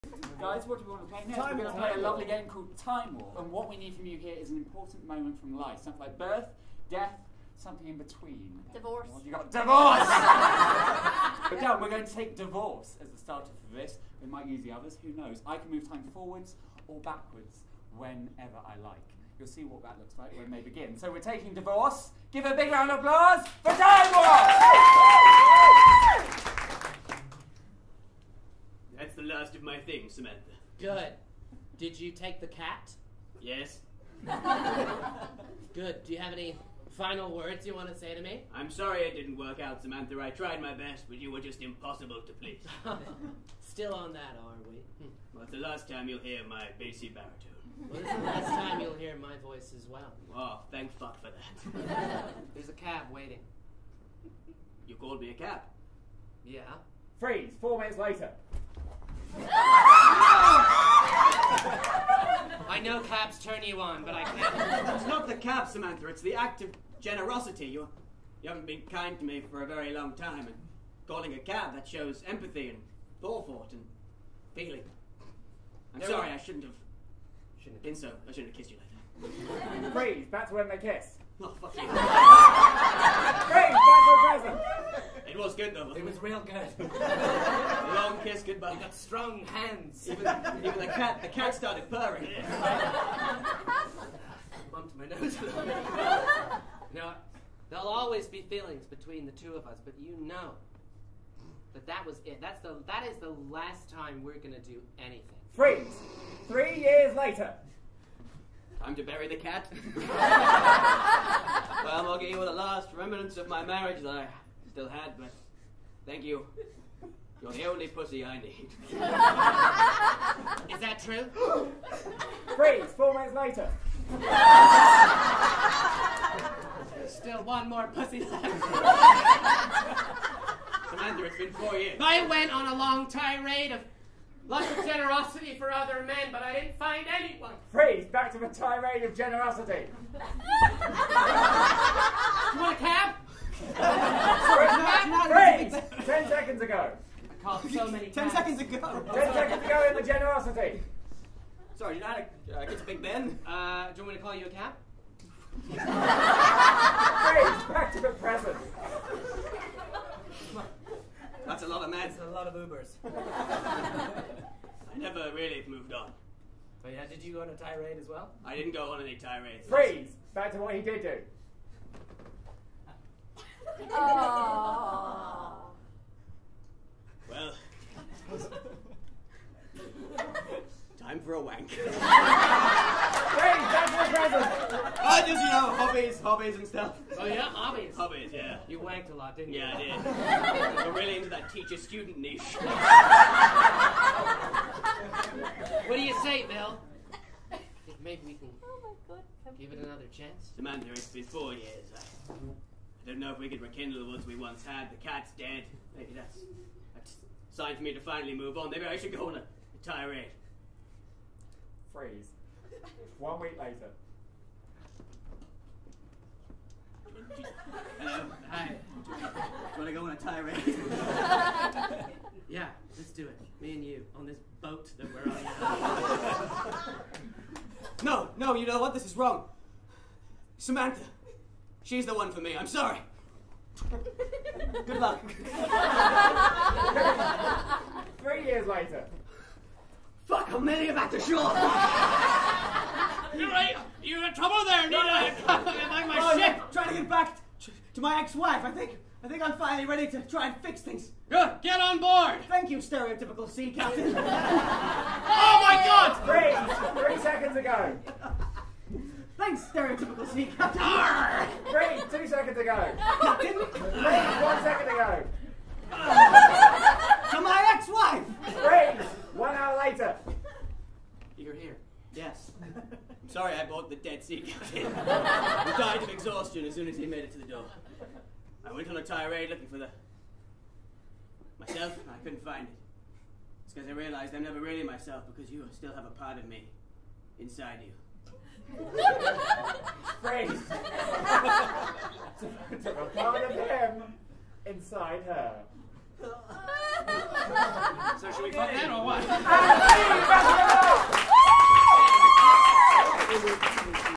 They can move time forwards or backwards... TIME WARP - DIVORCE
Shoot From The Hip play the improv game, TIME WARP. Suggestion was DIVORCE.

Credits:
-Chinese subtitles by Lijing
-English transcribed by Rebecca-Rose / subtitled by Imola
-Danish subtitles by Julie D.K.
-Dutch subtitles by Dani
-German subtitles by Franzi
-Spanish subtitles by Anna Day